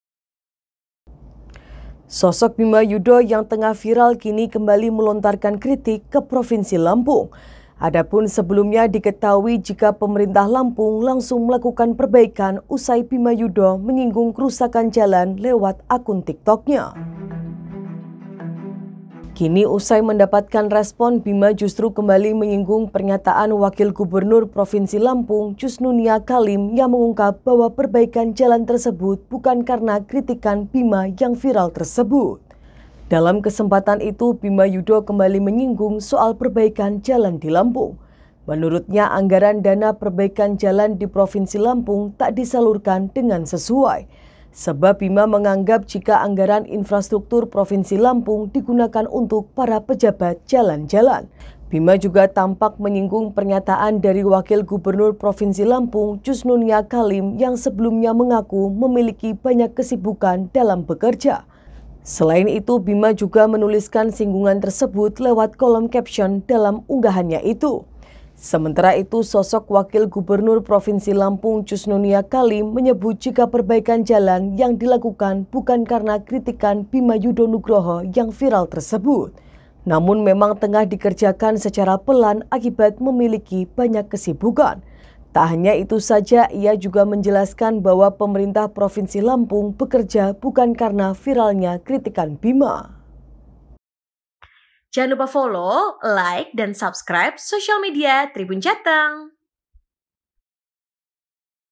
Tiktoker Bima Yudho Kembali Kritik Lampung, Sebut Anggaran Infrastruktur Dipakai Jalan Jalan
Sosok Bima Yudho yang tengah viral kini kembali melontarkan kritik ke Provinsi Lampung.

Sebelumnya diketahui jika pemerintah Lampung langsung melakukan perbaikan usai Bima Yudho menyinggung kerusakan jalan lewat akun TikToknya @awbimaxreborn.

Kini, usai mendapatkan respon, Bima justru kembali menyinggung pernyataan Wakil Gubernur Provinsi Lampung Chusnunia Chalim yang mengungkap bahwa perbaikan jalan tersebut bukan karena kritikan Bima Yudho Nugroho yang viral tersebut.